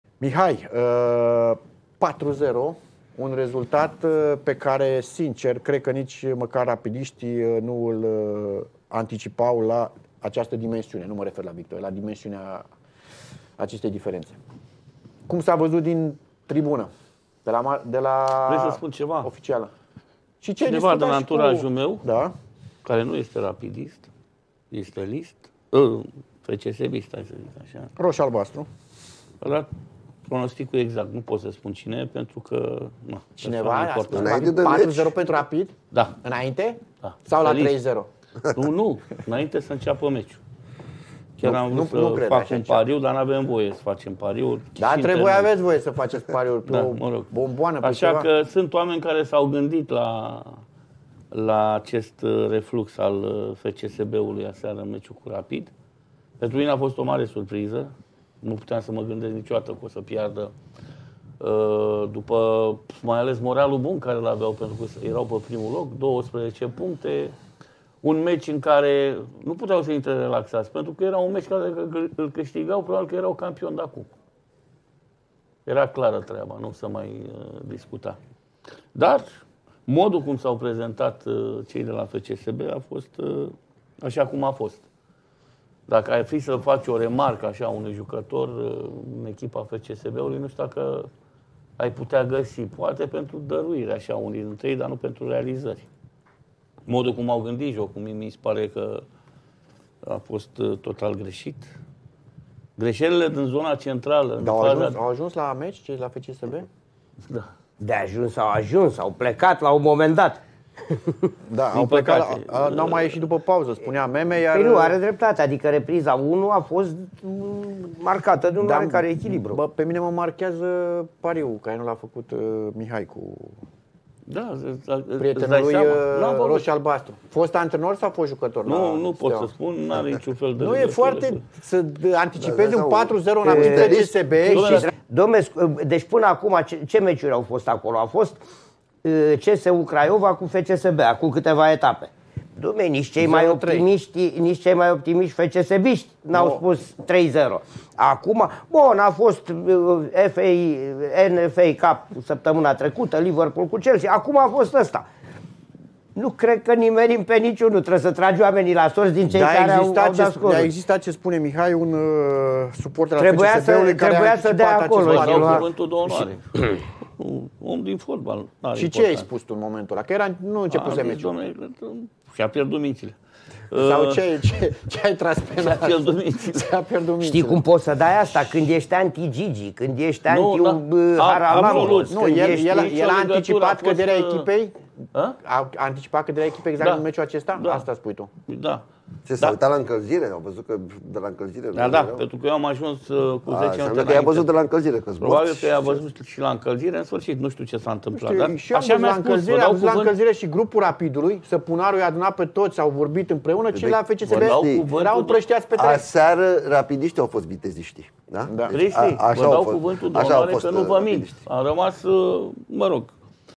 Mihai Stoichita, DEZVALUIRE SOC: “La tribuna VIP S-A GHICIT SCORUL CORECT!” Rapid - FCSB 4-0
In direct la FANATIK SuperLiga, Mihai Stoichita a dezvaluit ca unul dintre apropiatii sai a ghicit scorul corect la derby-ul Rapid - FCSB 4-0. Cine e apropiatul lui Mihai Stoichita care a nimerit scorul exact la meciul Rapid - FCSB